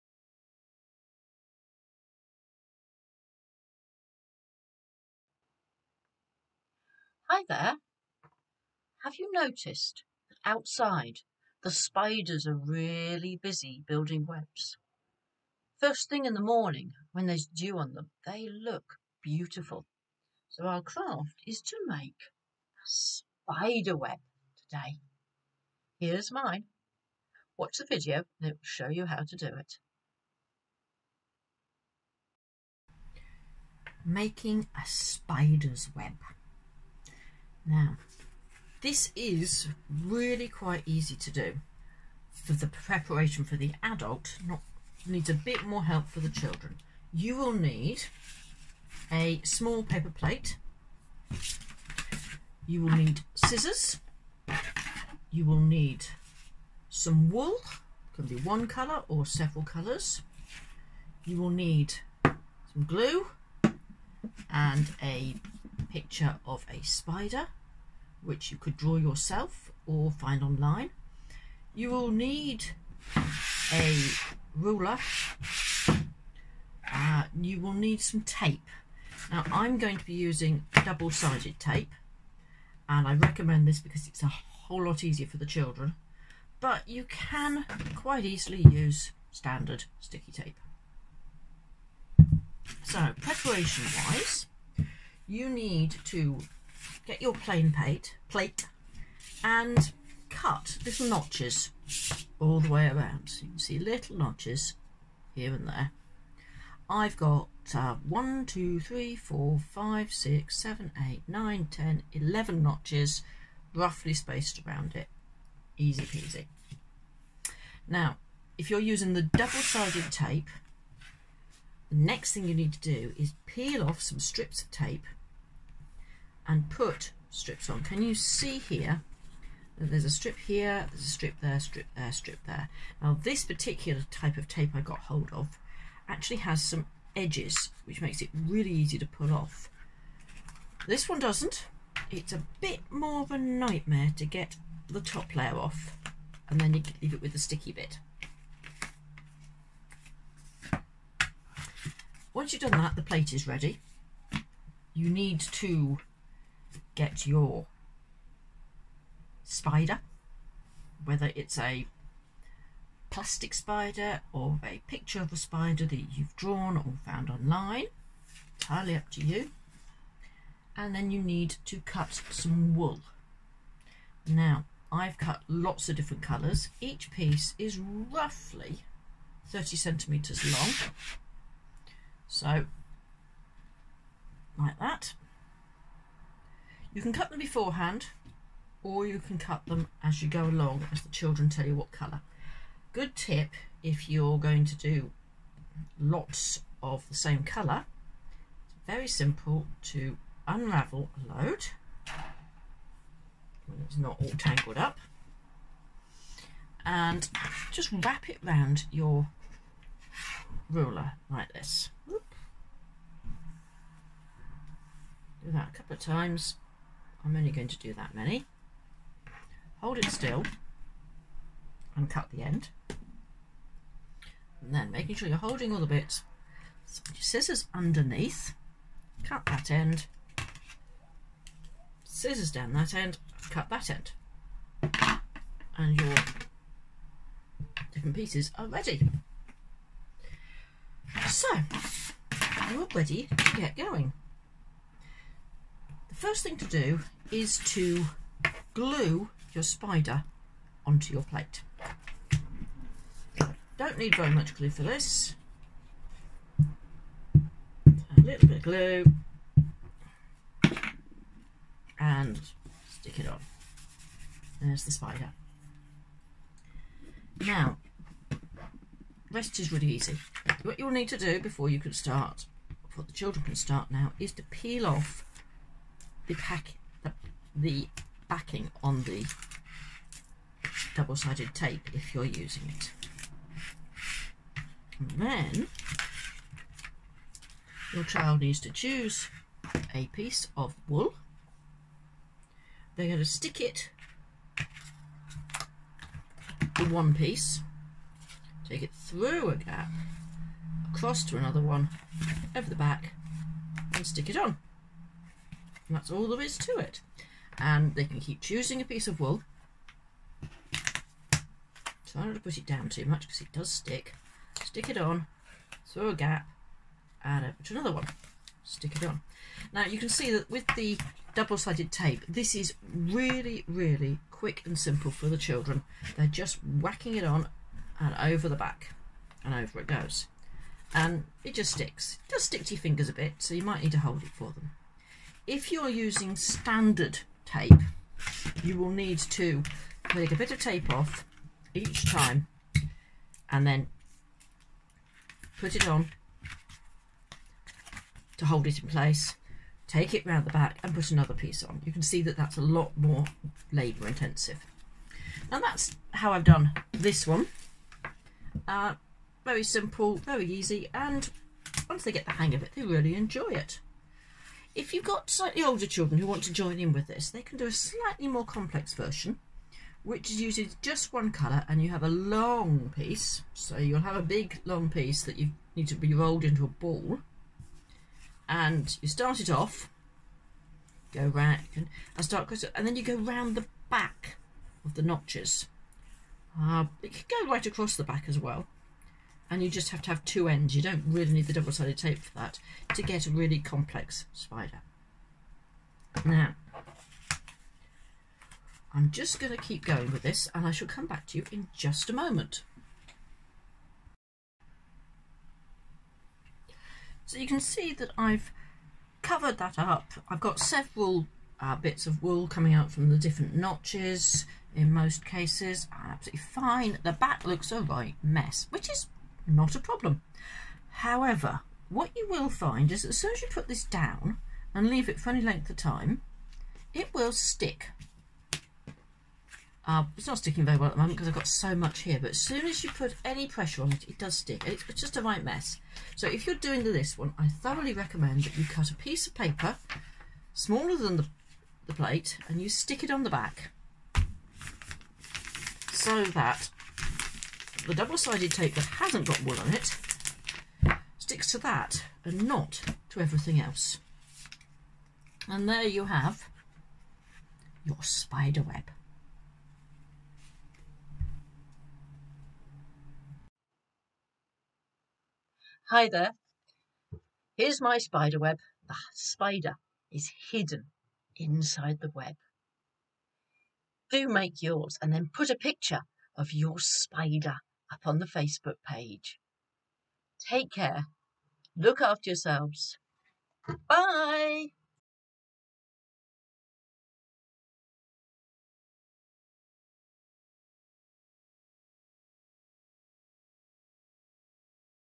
Spider web
Make your own spider web model.  You will need: small paper plate, wool, tape (double-sided is best), scissors, glue and a spider picture or model.